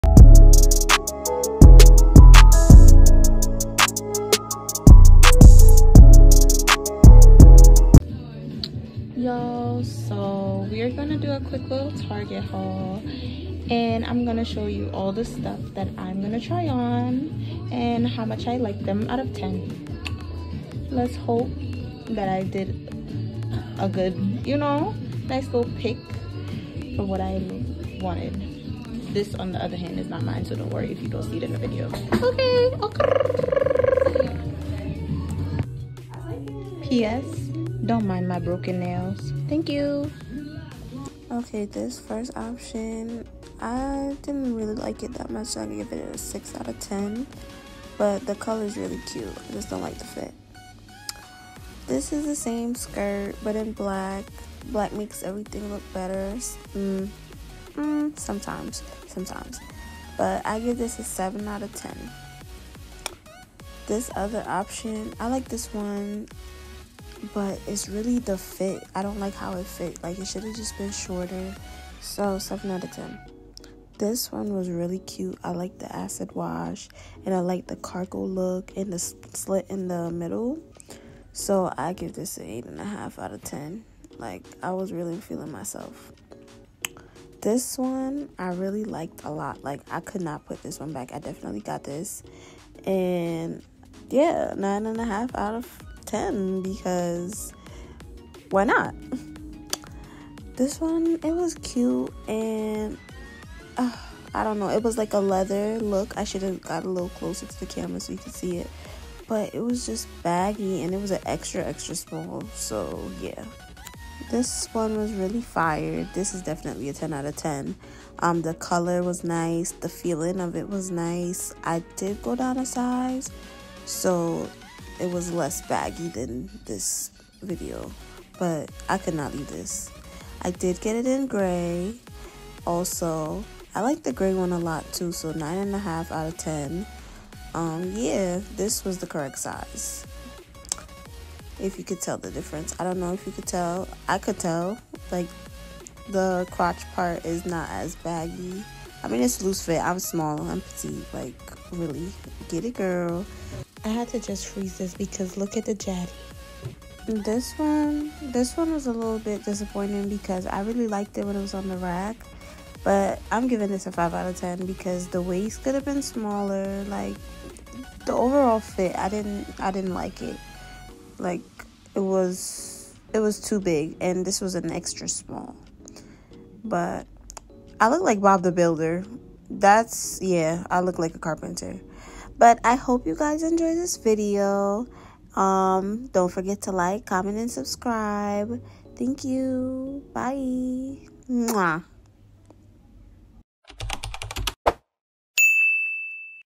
Mini Target Clothing Haul 🛍️(Bottoms) w/ REVIEWS
I found some fire 🔥 pieces in target. Watch me rate them and show you what I bought🛍️. LET ME PUT YOU ONNN!

Purple skirt

Black skirt

Beige skirt

Acid wash cargo skirt

Cargo jeans pants

Beige Leather Pants (Sold out at target)

Green parachute cargo pants

Grey parachute cargo pants

Beige Tube Cargo Jumpsuit

Purple skirt

Black skirt

Beige skirt

Cargo jeans pants